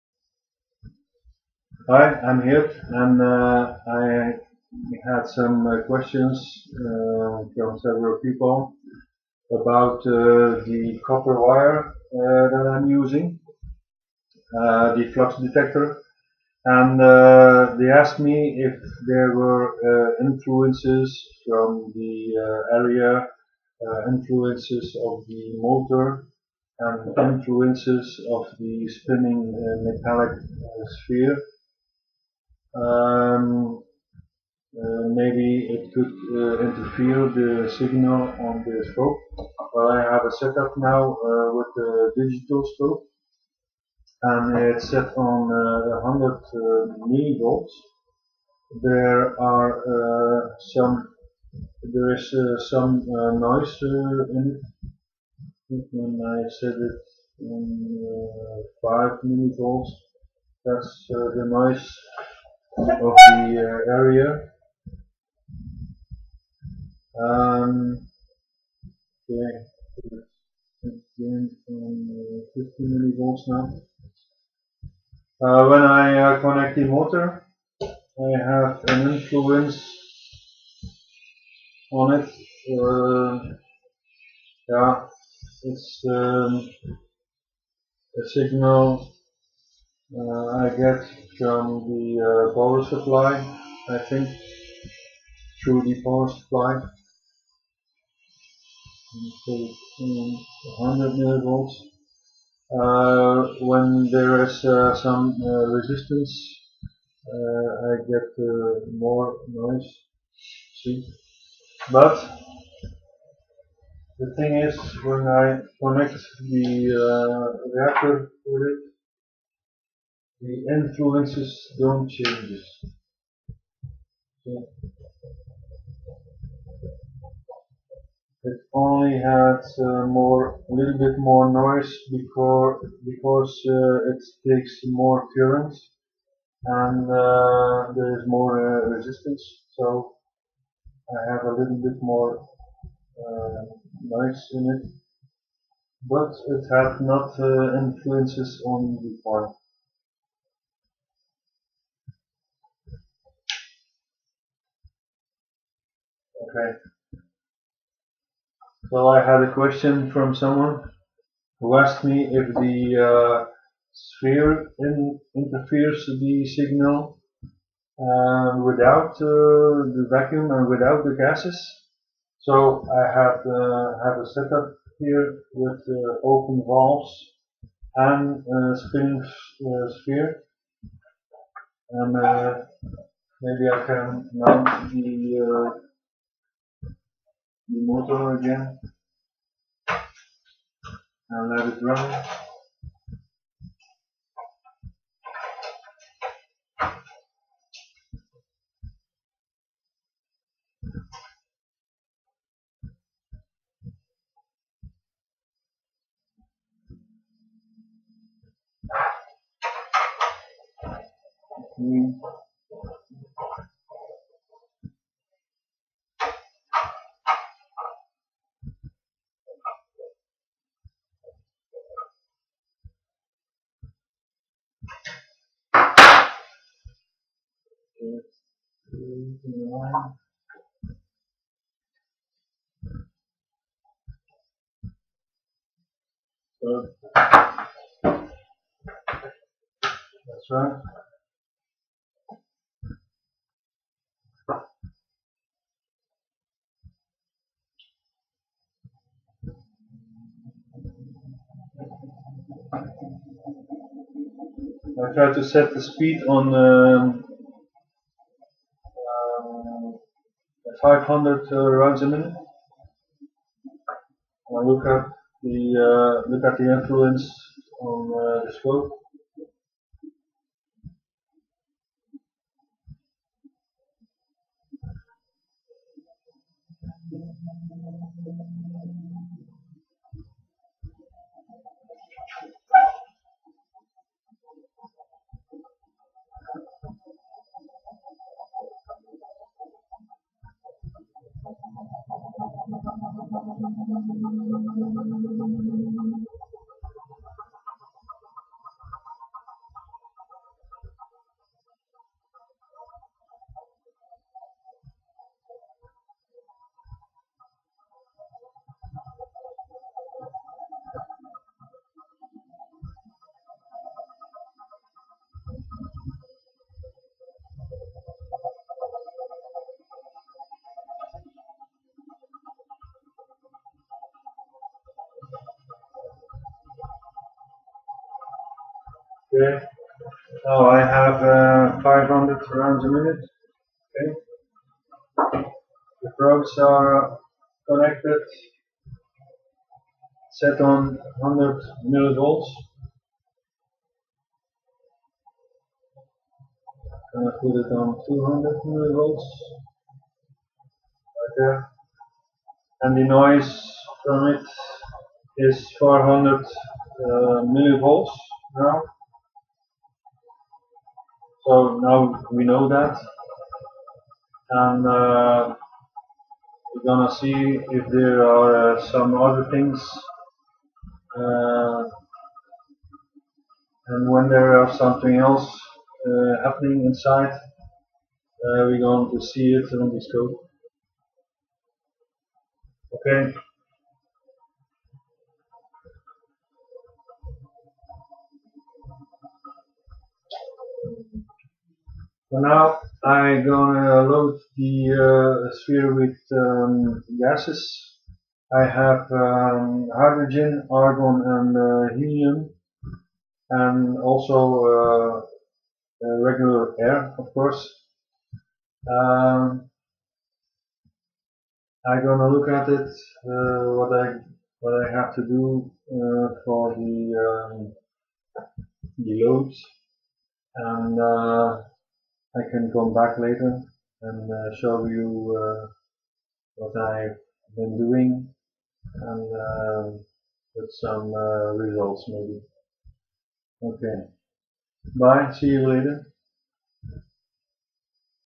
V11_Noise tests 2013 12 07 14 43
I try to answer through this test a few questions. The copper coil detects environmental and other influences. Test with only a spinning sphere, without gas or vacuum.